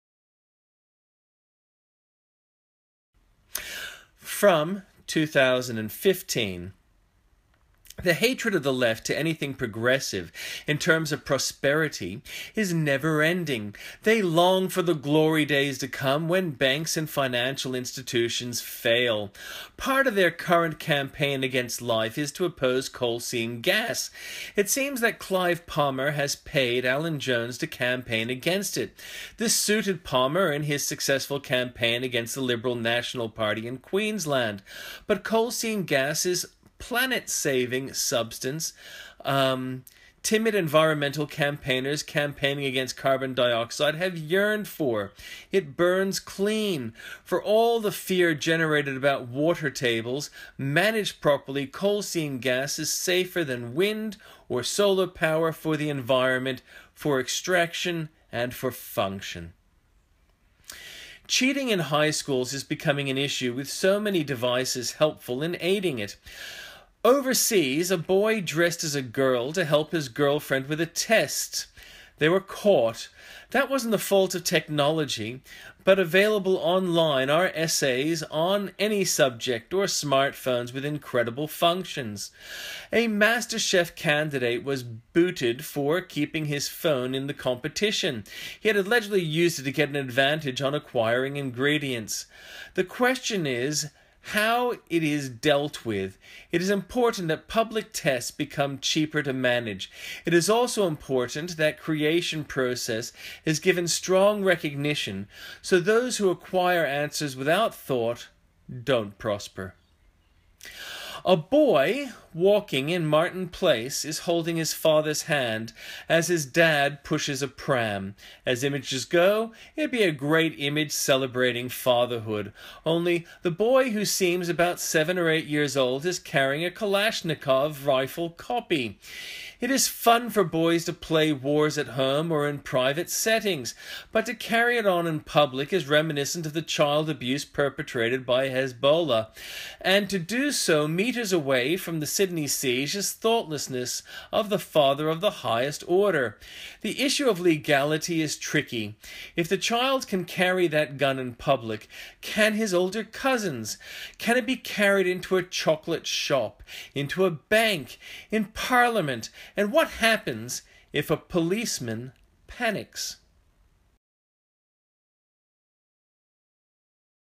Jun 11th 2015
The hatred of the left to anything progressive in terms of prosperity is never ending. They long for the glory days to come when banks and financial institutions fail. Part of their current campaign against life is to oppose coal seam gas. It seems that Clive Palmer has paid Alan Jones to campaign against it. This suited Palmer in his successful campaign against the LNP in Queensland. But coal seam gas is the 'planet saving' substance timid environmental campaigners campaigning against Carbon Dioxide have yearned for. It burns clean. For all the fear generated about water tables, managed properly, Coal Seam Gas is safer than wind or solar power for the environment, for extraction and for function.

Cheating in high schools is becoming an issue with so many devices helpful in aiding it. Overseas, a boy dressed as a girl to help his girlfriend with a test. They were caught. That wasn't the fault of technology, but available online are essays on any subject, or smart phones with incredible functions. A Masterchef candidate was booted for keeping his phone in the competition. He had allegedly used it to get an advantage on acquiring ingredients. The question is how it is dealt with. It is important that pubic tests become cheaper to manage. It is also important that the creation process is given strong recognition, so those who acquire answers without thought don't prosper.

A boy walking in Martin Place is holding his father's hand as his dad pushes a pram. As images go it would be a great image celebrating fatherhood. Only the boy, who seems about seven or eight years old is carrying a Kalashnikov Rifle copy. It is fun for boys to play wars at home or in a private setting, but to carry it in public is reminiscent of the child abuse perpetrated by Hezbollah. And to do so metres away from the Sydney Siege is thoughtlessness of the father of the highest order. The issue of legality is tricky. If the child can carry that gun in public, can his older cousins? Can it be carried into a chocolate shop? Into a bank? Into parliament? And what happens if a policeman panics?

videos by David Daniel Ball for the Conservative Voice Author of History in a Year by the Conservative Voice